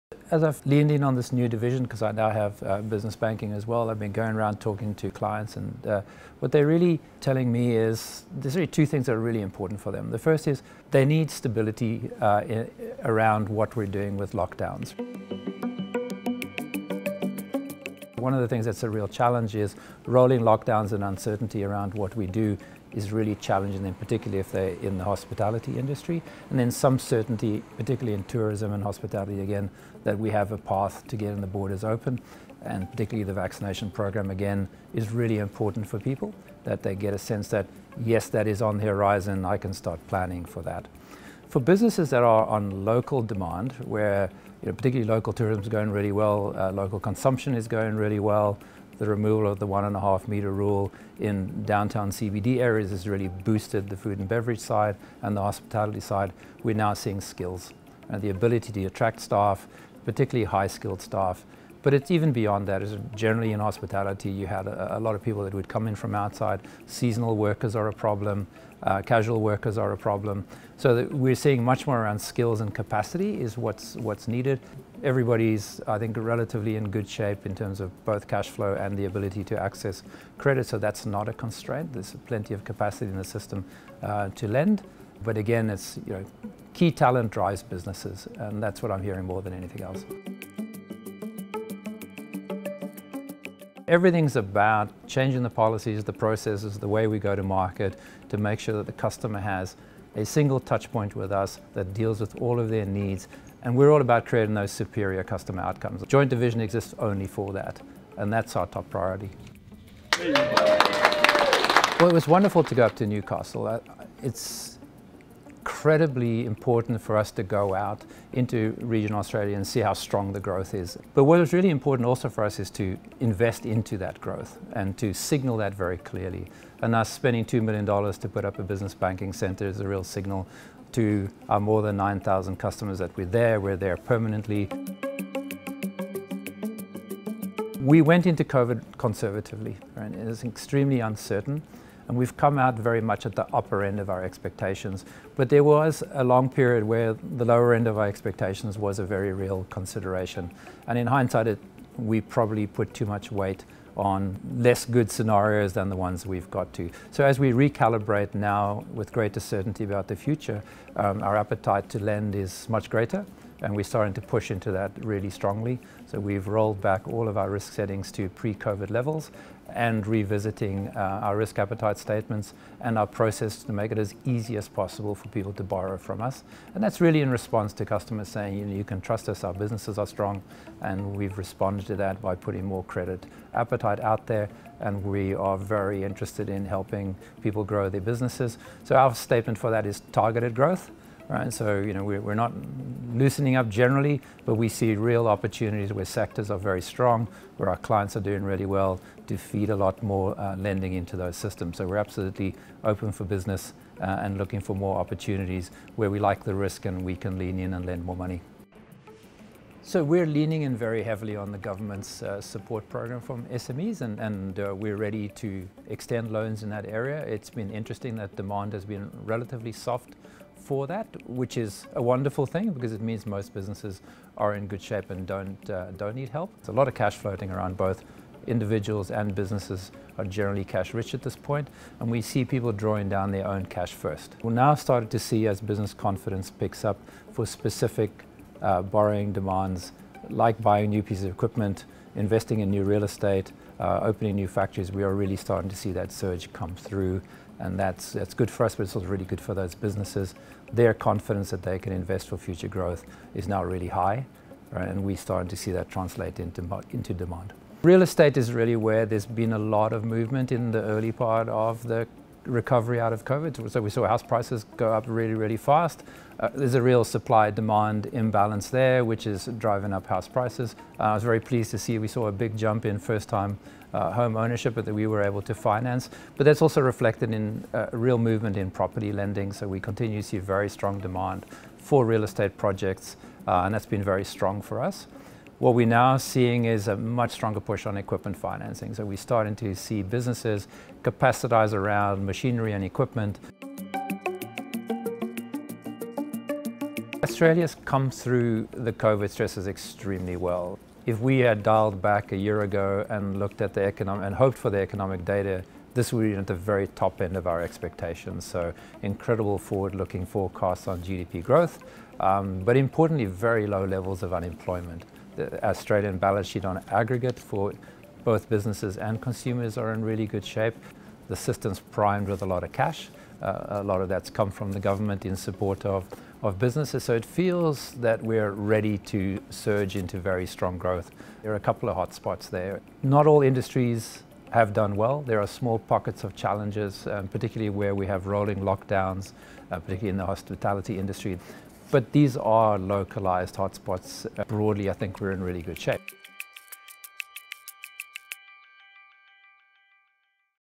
'Really good shape': Chris de Bruin upbeat on economy, loan demand
Fresh from visiting the opening of Westpac’s new Newcastle business banking centre, Chris de Bruin, Chief Executive, Consumer & Business Banking, discusses the economy, loan demand, his new division and COVID-19.